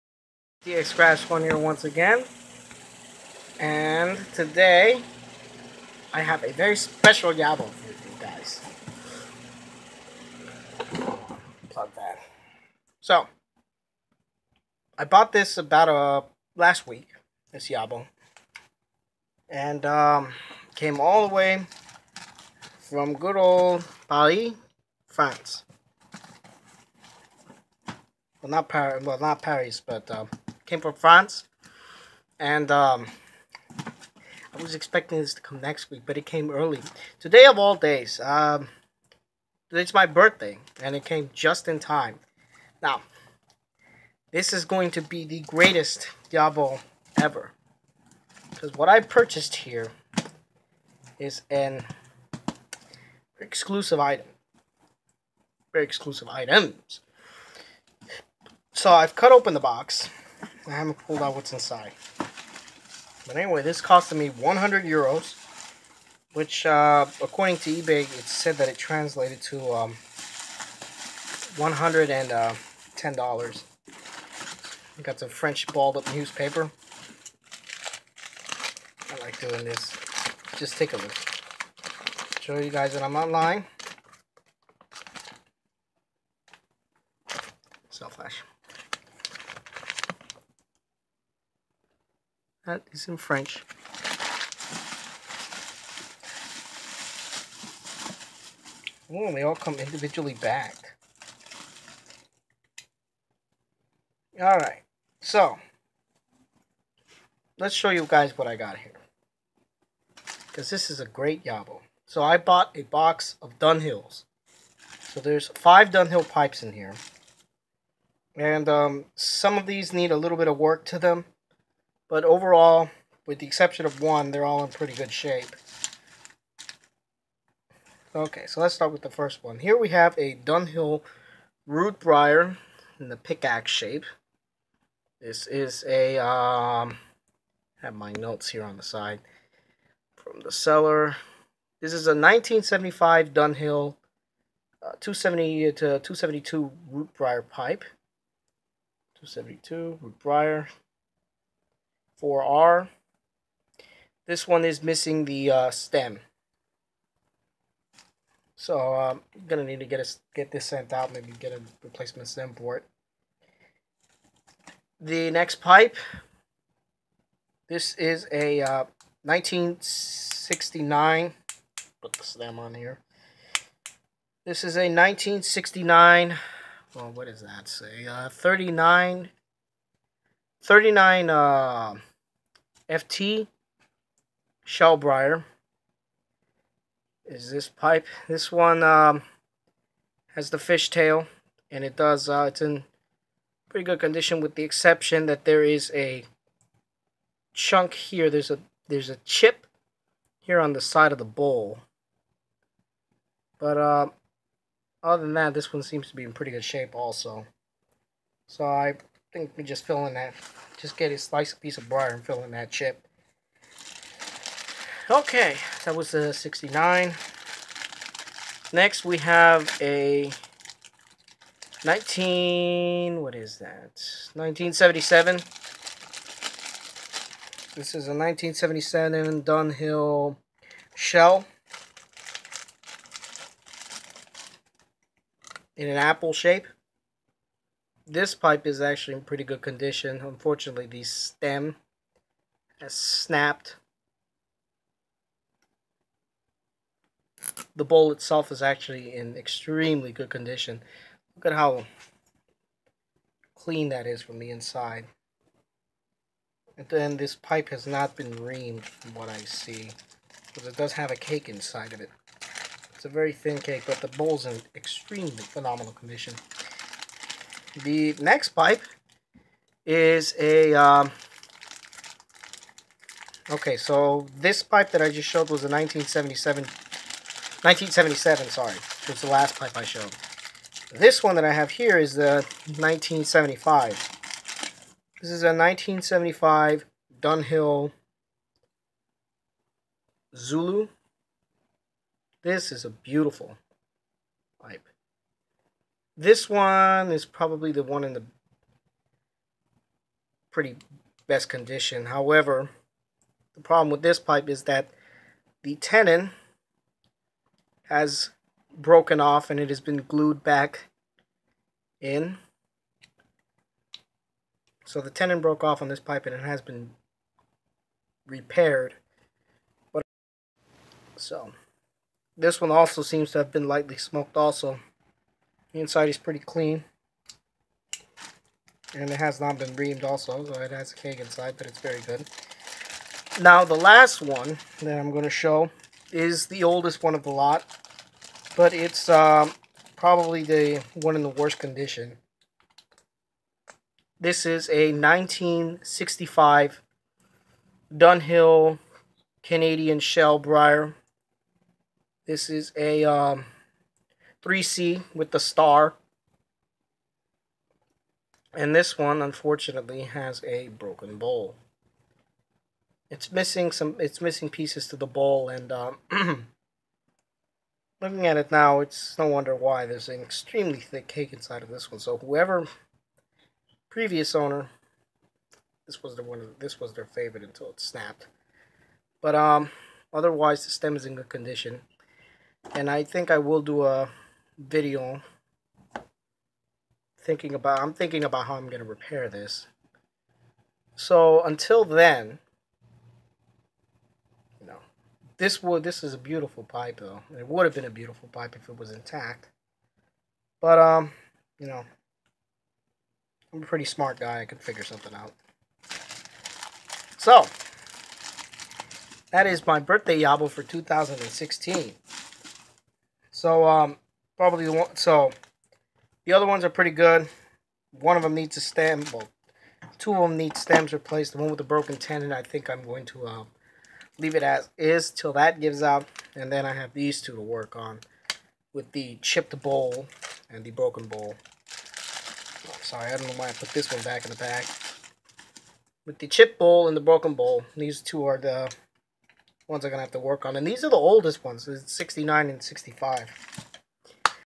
Birthday Yabo From France 2016
Lot of 5 Dunhill's in time for my birthday